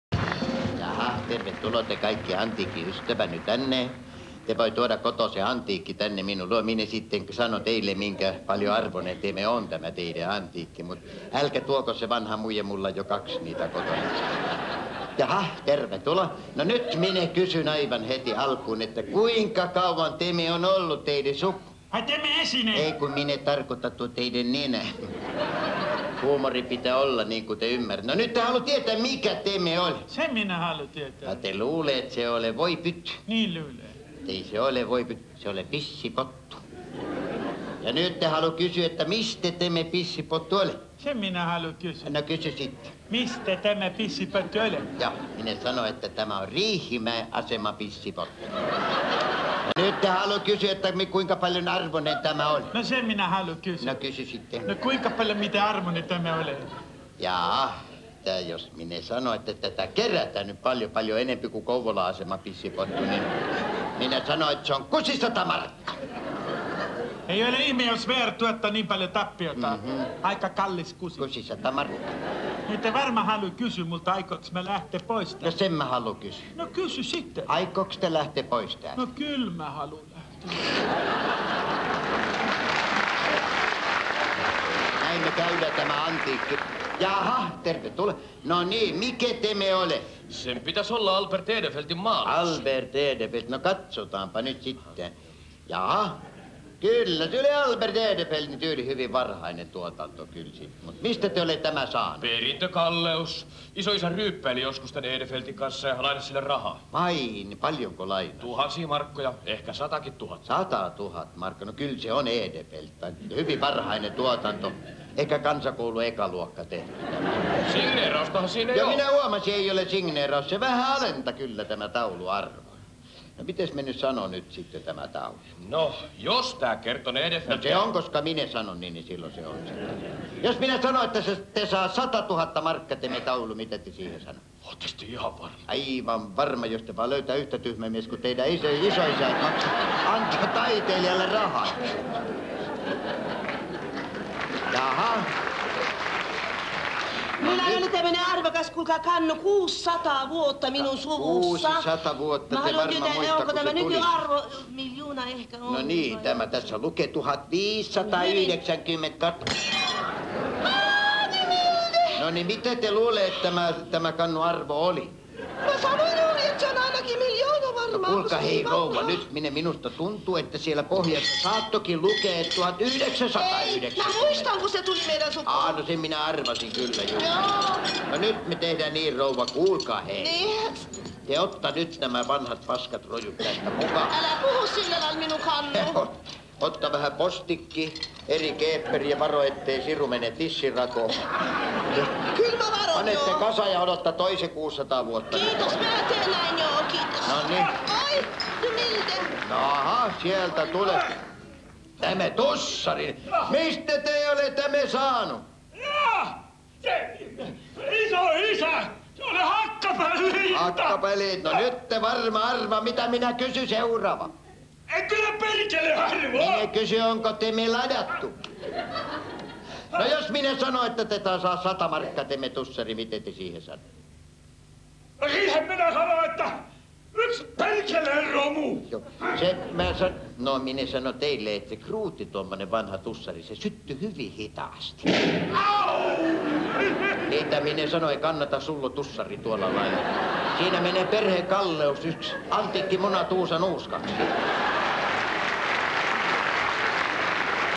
Antiikkia, antiikkia -parodia (1998)
Komediateatteri Arenan revyyn sketsi parodioi "Antiikkia, antiikkia" tv-ohjelmaa, jossa suomenruotsalainen Wenzel Hagelstam arvioi tavisten vanhojen esineiden arvoa.
Rooleissa: Heikki Kinnunen, Fredi, Toni Wahlström, Kristiina Elstelä ja Eero Melasniemi. Ohjaus: Ere Kokkonen.
Komediateatteri Arena: "Torvensoittaja katolla revyy" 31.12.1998